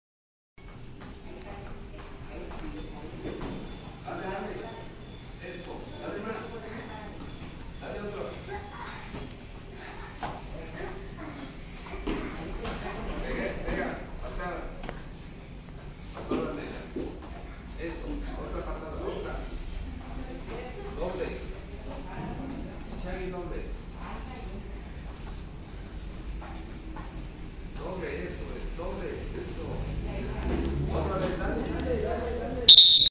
EL COMBATE MAS VIOLENTO DE ACADEMIA ELITE DE TKD
ESTE COMBATE SE LLEVÓ A CABO EN MAYO 2012
BRENDA VS LEYLA
NO APTO PARA CARDIACOS!!